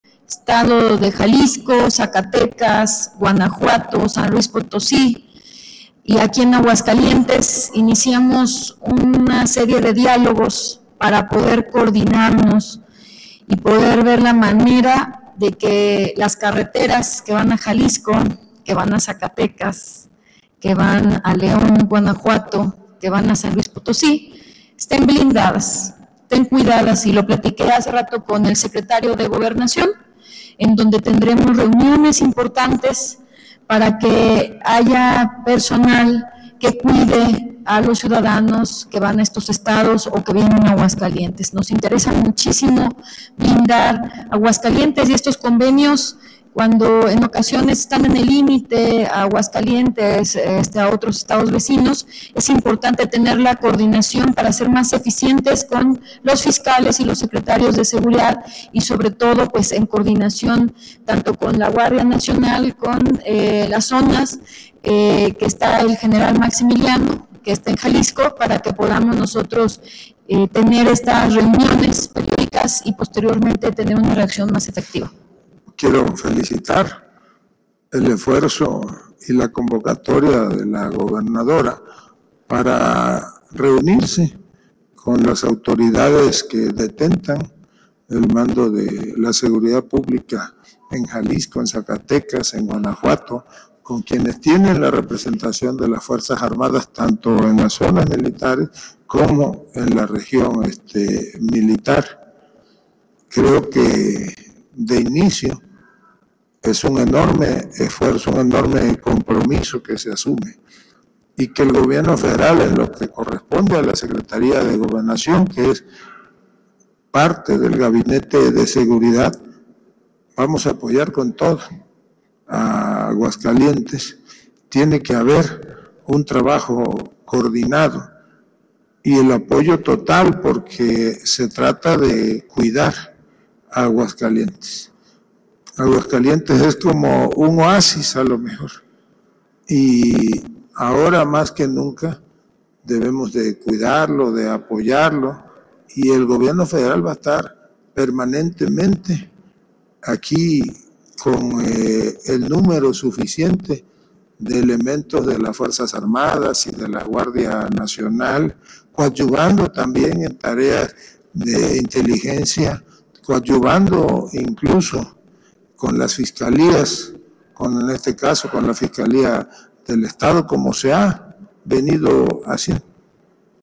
“Felicito el esfuerzo de la Gobernadora Tere Jiménez por mantener la paz en Aguascalientes”: Augusto
Aguascalientes, Ags, 3 noviembre 2022.- Luego que esta misma semana la gobernadora Tere Jiménez Esquivel encabezara una reunión interestatal encamina a sumar esfuerzos entre los gobiernos de Aguascalientes, Jalisco y Zacatecas, así como las fuerzas armadas federales para blindar las carreteras que conectan las tres entidades, el Secretario de Gobernación, Adán Augusto López Hernández felicitó el esfuerzo y la convocatoria de la mandataria estatal por mantener la seguridad.
Ofreció todo el apoyo del Gobierno de la República para que Aguascalientes se mantenga como un oasis de seguridad en el territorio nacional y hoy más que nunca debe apoyársele con el número suficiente de las fuerzas armadas y de la Guardia Nacional, así como con tareas de inteligencia.